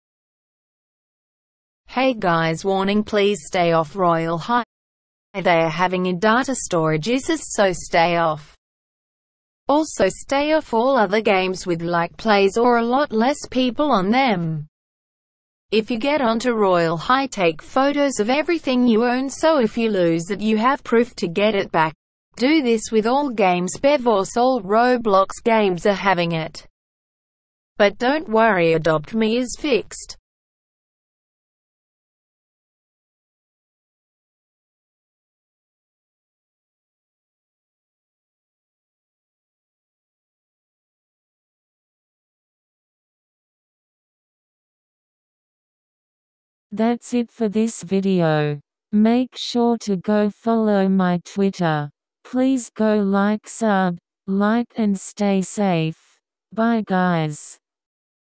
⚠️Royale High getting deleted!? Stay off roblox!⚠️
Please watch this is so important. I made this video at my own risk of losing my items